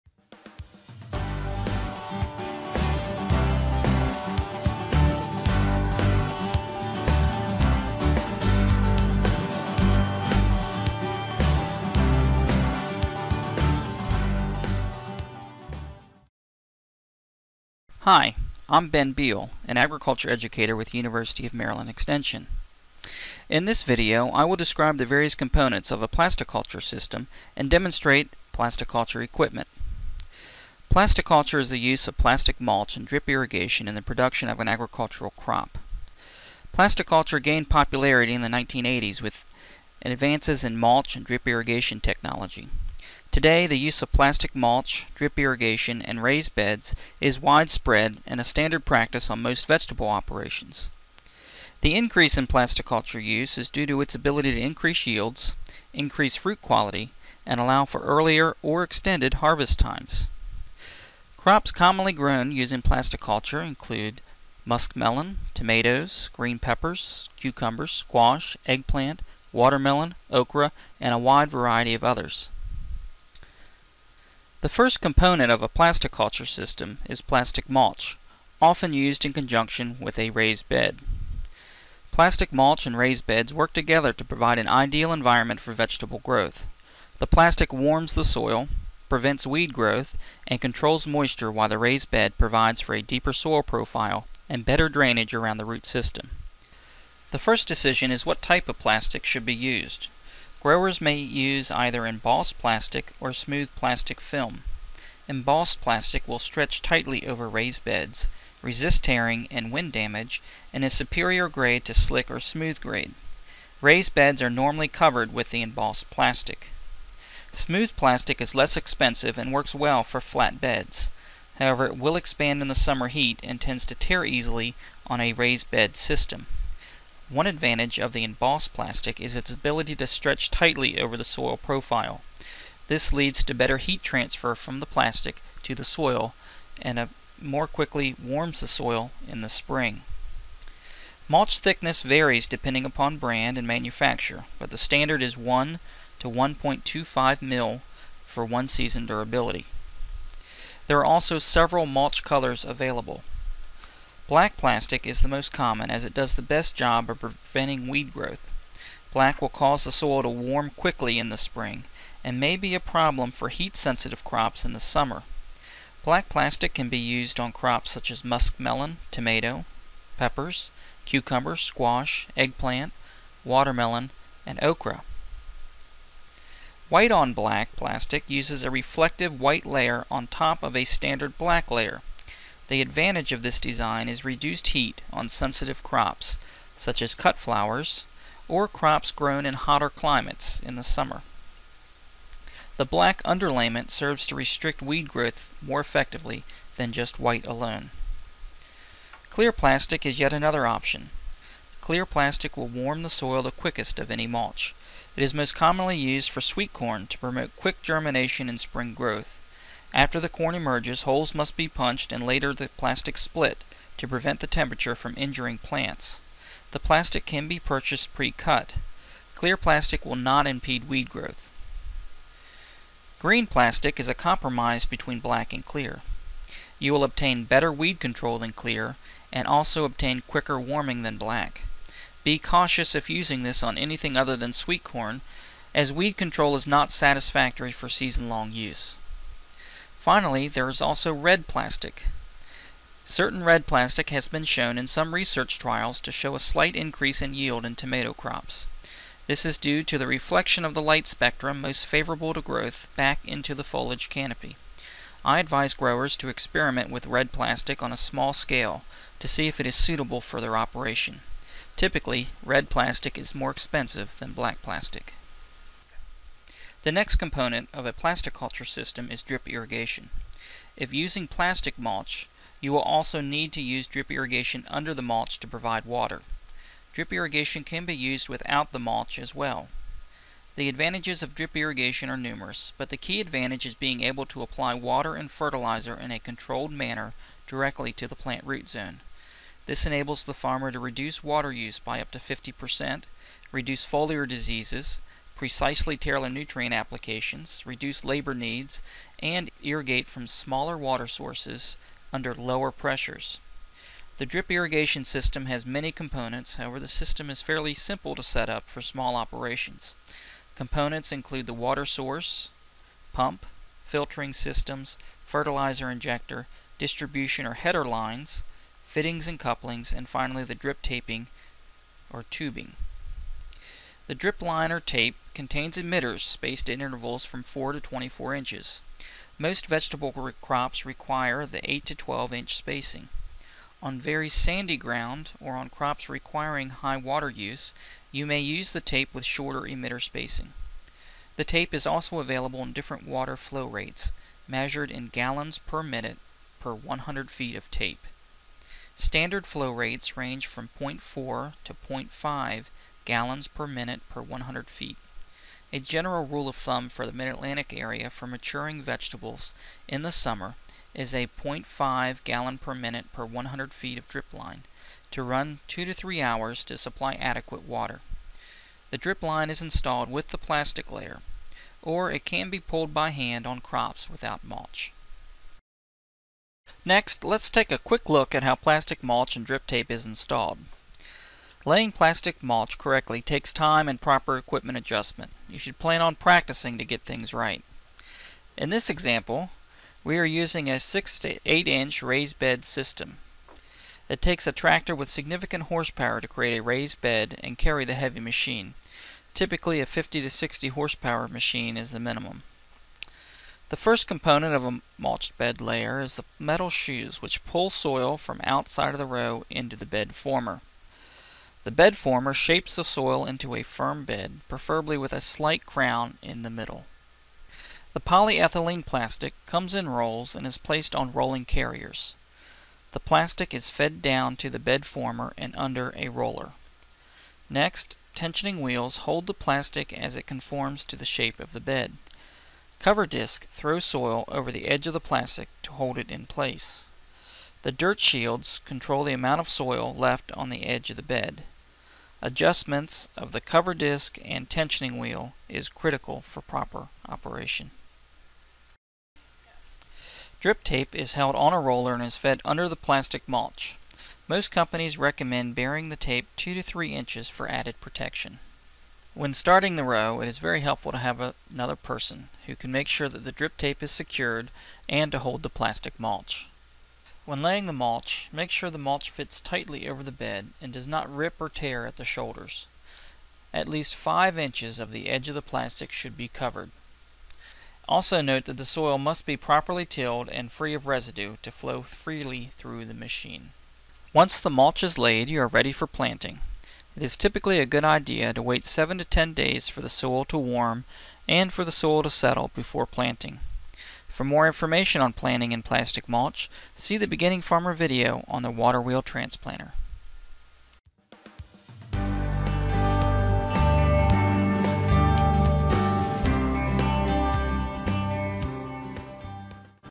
Introduction to Plasticulture and Drip Irrigation
University of Maryland Extension Educator, Ben Beale, shares information about plastic mulch and demonstrations in applying plastic mulch to raised soil beds.